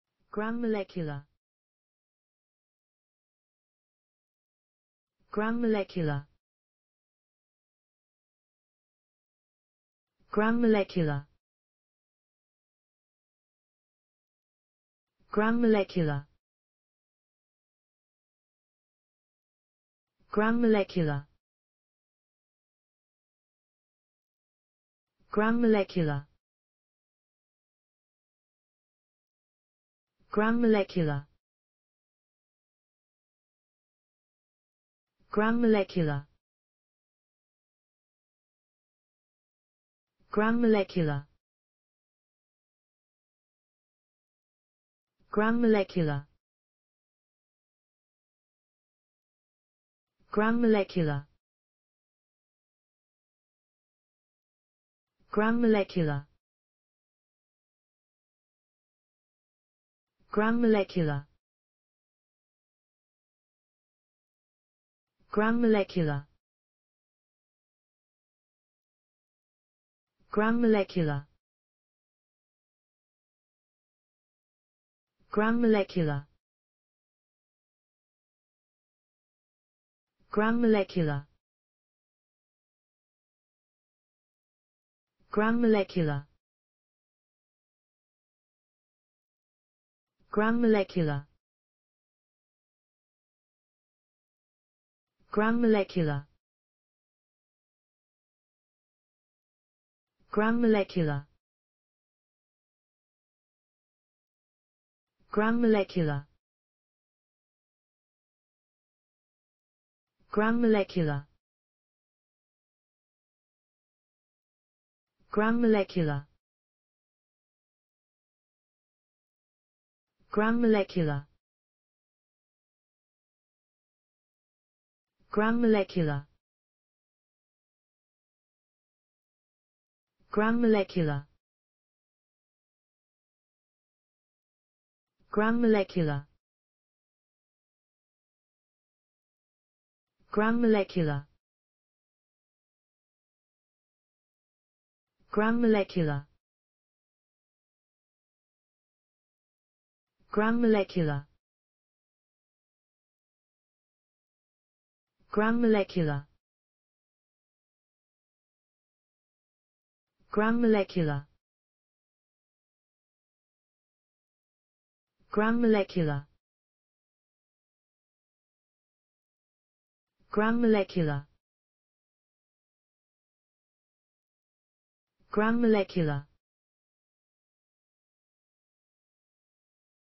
Gram-molecular. How to pronounce the english word Gram-molecular .Start with G. Learn from me.
Follow me!Learn from me how to pronounce the english word Gram-molecular.This video shows you how to pronounce the English word Gram-molecular .